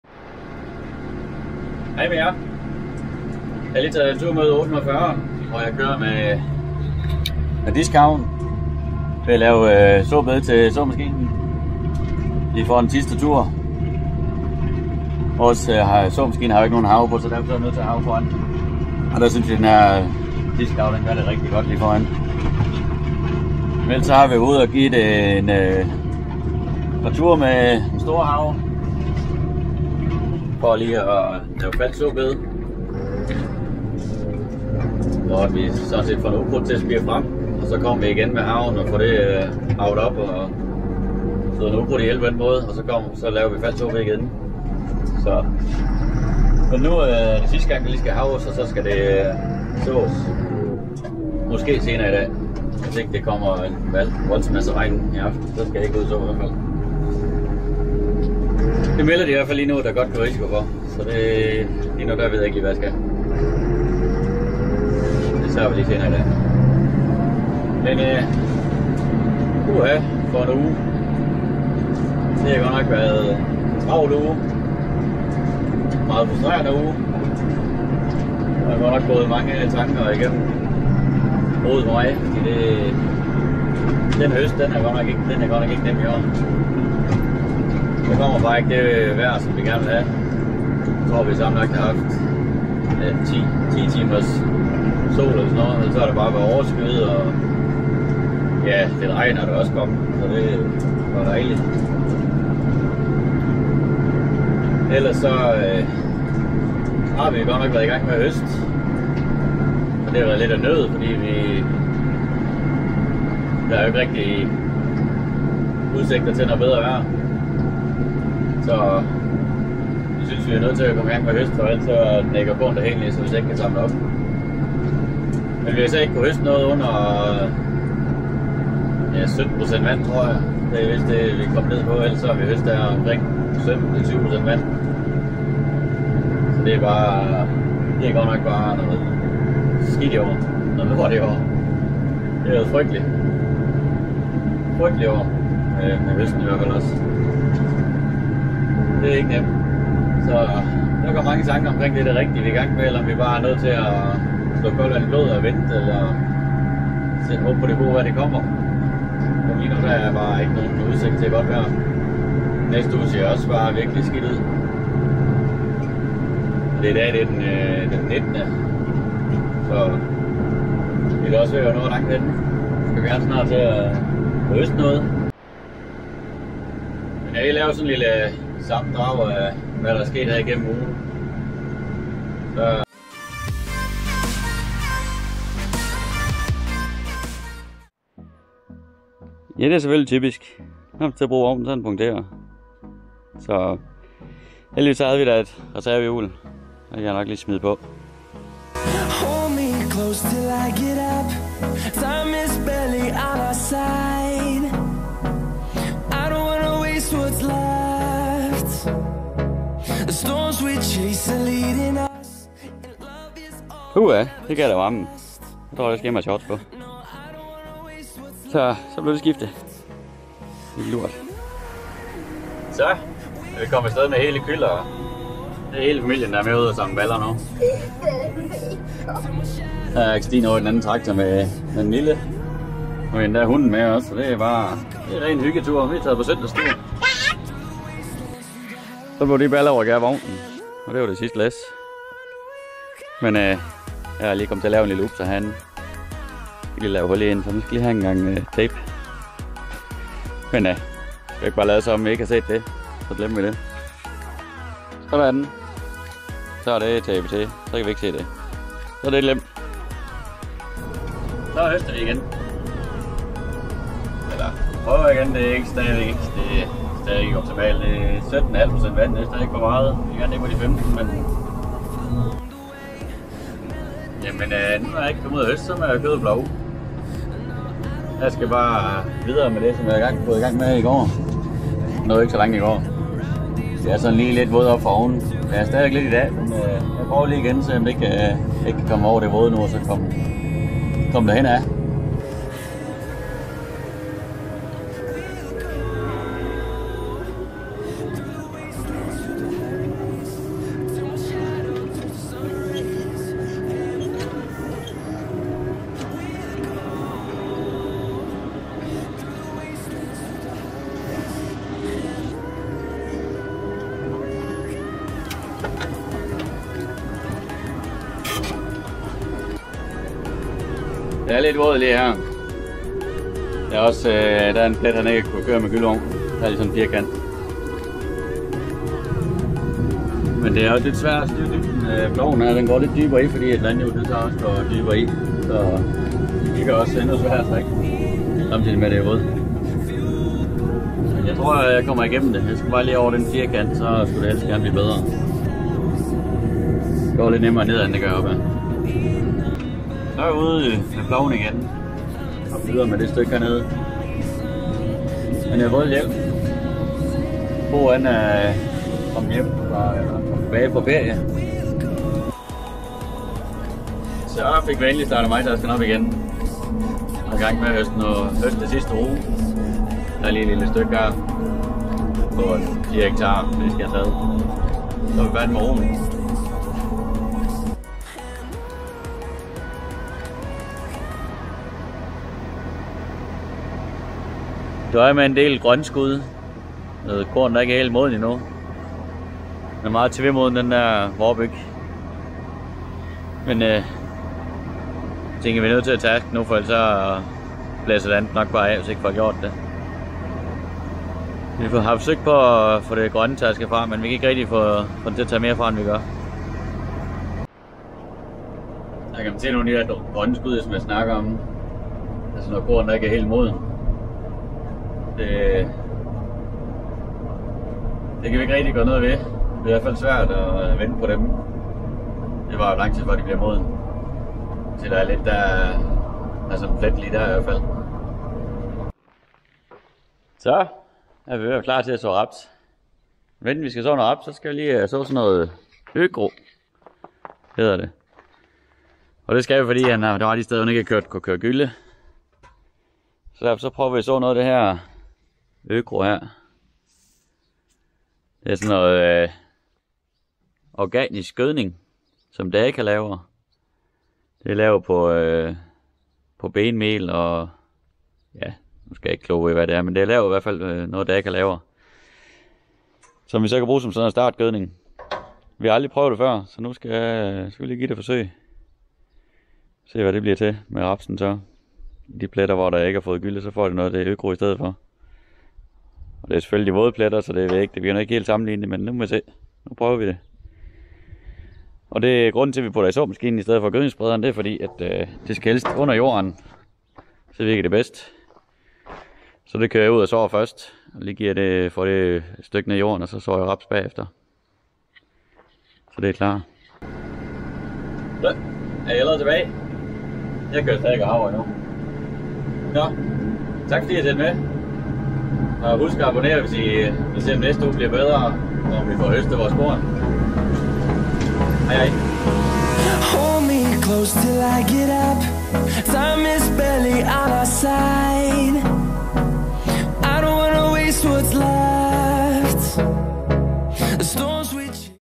Frygtelig frustrerende høst uge..
Så gik der endnu en uge, men det har været en hård og træls uge med nærmest ingen høstvejr. Lad os håbe det bliver bedre i næste uge.